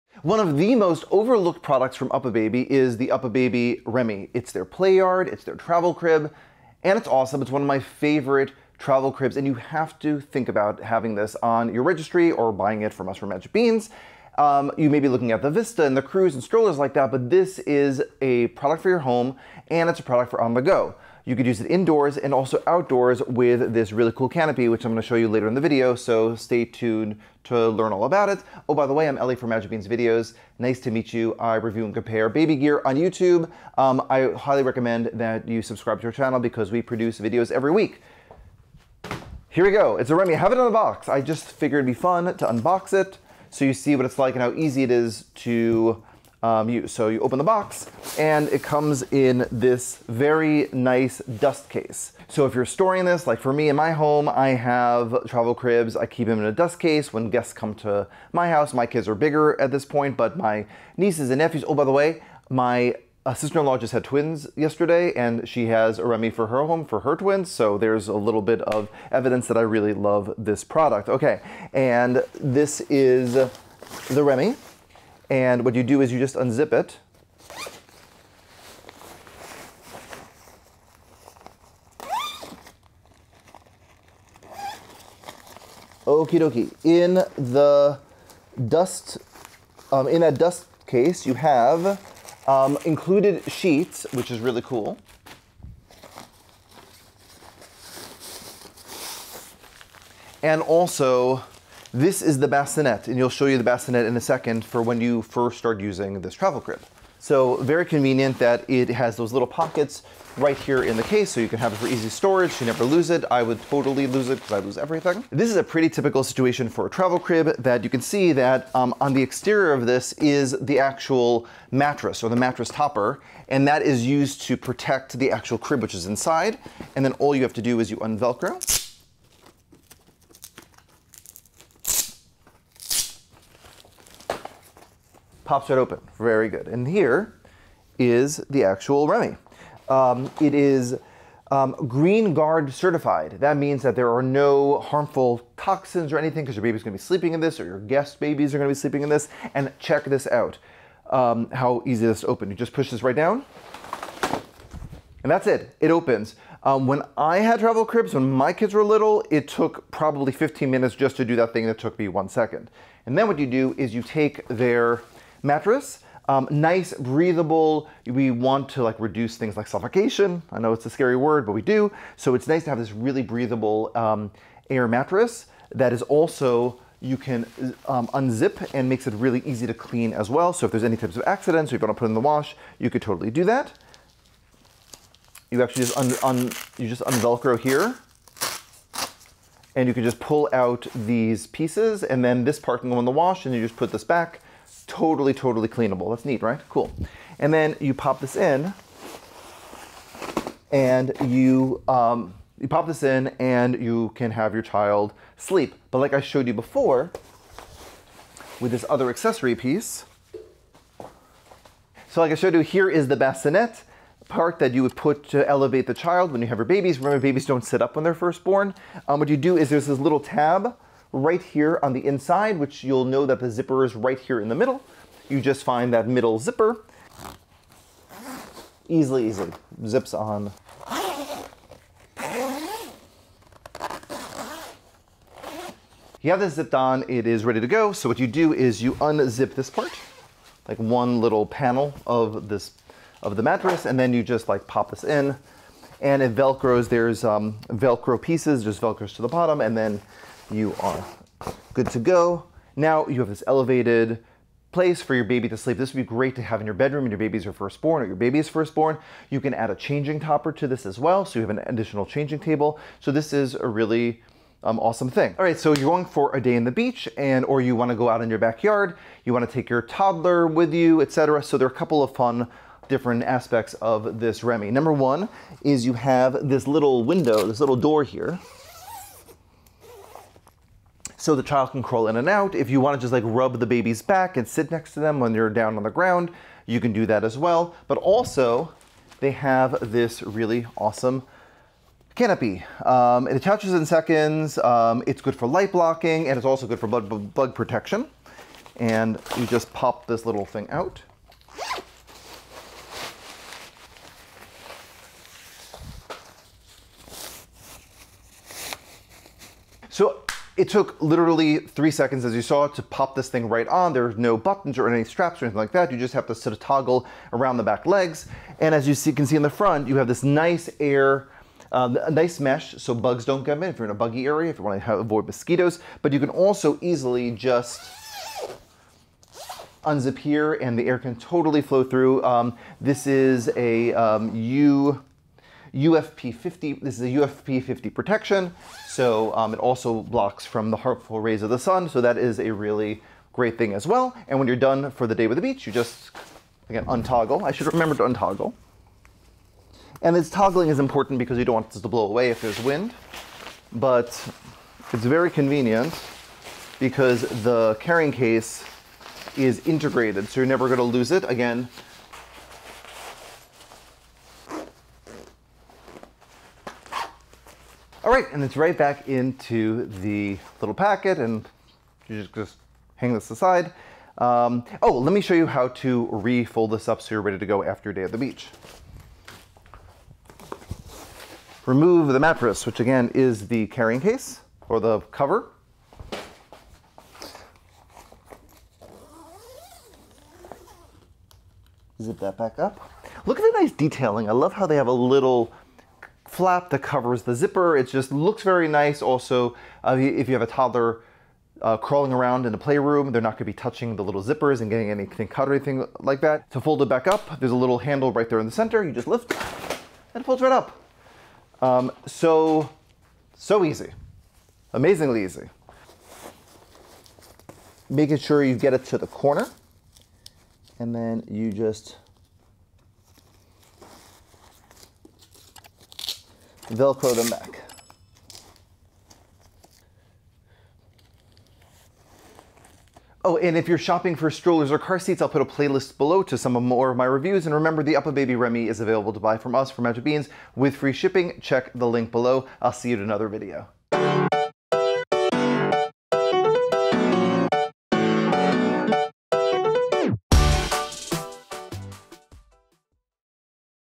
UPPAbaby REMI | Best Travel Cribs | Magic Beans Reviews
In this video, Eli reviews the UPPAbaby Remi Travel Crib/ Play-yard. UPPAbaby is known for their super popular Vista stroller and have recently added the Remi to their assortment. The Remi is multi-functional and offers some really exciting features. The Remi can be used as a bedside bassinet from birth to 6 months, UPPAbaby even offers a changing station that can be added to the system. The Remi can also be used as a travel crib and play yard. The Remi is GREENGUARD certified and features a breathable, machine washable mattress. It provides easy access with a zipfront "door." UPPAbaby also offers a canopy that allows parents to use the Remi outside. The Remi canopy provides sun protection and keeps bugs and mosiquotos out.

Any questions? We'll match you with the perfect baby gear for your family and lifestyle.